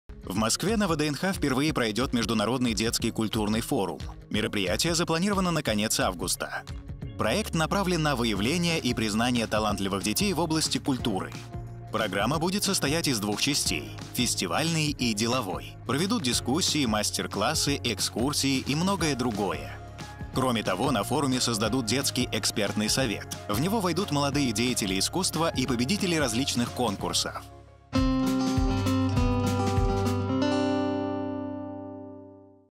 МЕЖДУНАРОДНЫЙ ДЕТСКИЙ КУЛЬТУРНЫЙ ФОРУМ В МОСКВЕ
Международный детский культурный форум впервые пройдёт в Москве. Его цель — выявление и признание талантливых детей, проявивших способности в области культуры и искусства. В программе форума запланированы секции по направлениям, дискуссии, встречи с ведущими деятелями культуры, мастер-классы, экскурсии. Кроме того, на форуме создадут детский экспертный совет, куда пригласят молодых деятелей искусства и победителей различных конкурсов.